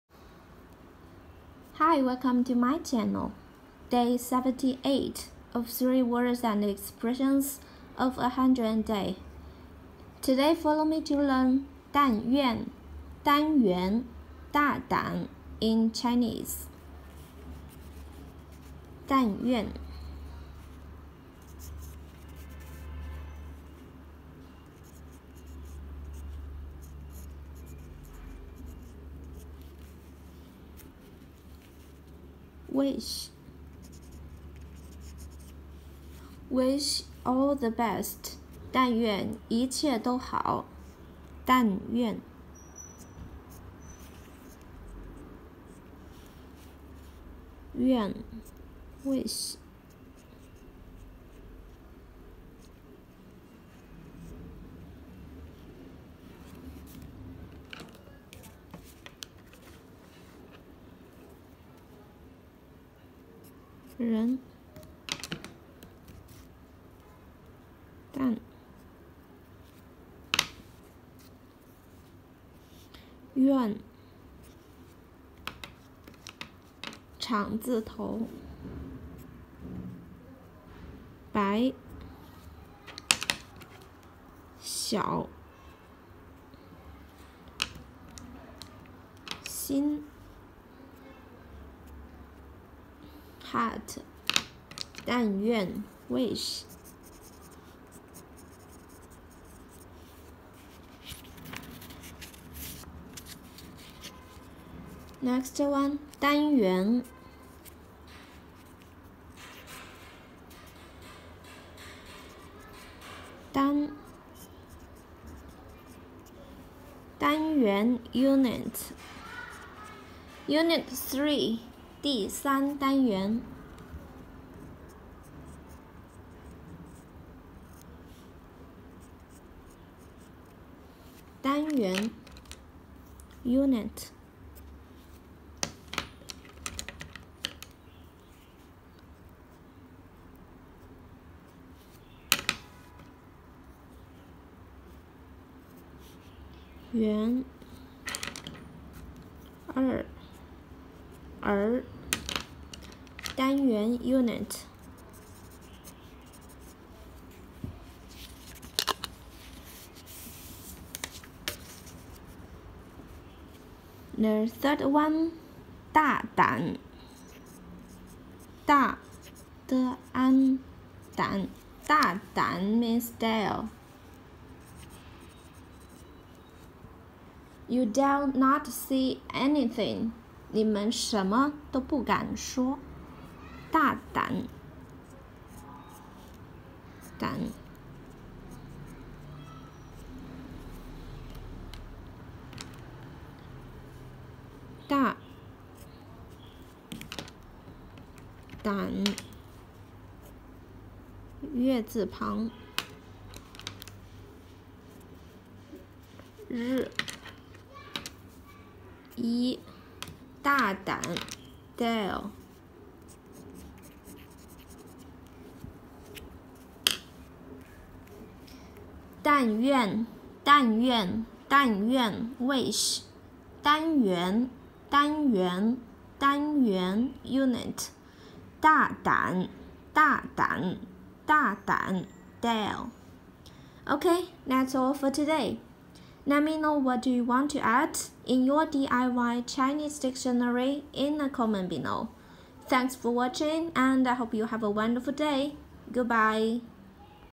DCD-Day 78-How to Write "wish/element/daring" in Chinese Characters
Hello Friends, in this episode follow me to DIY your own Chinese Dictionary.

Day 78 of "3 words and expressions of 100-day".

dàn yuàn 但愿 wish
dān yuán 单元 element
dà dǎn 大胆 daring

Let me know what do you want to learn in the comment!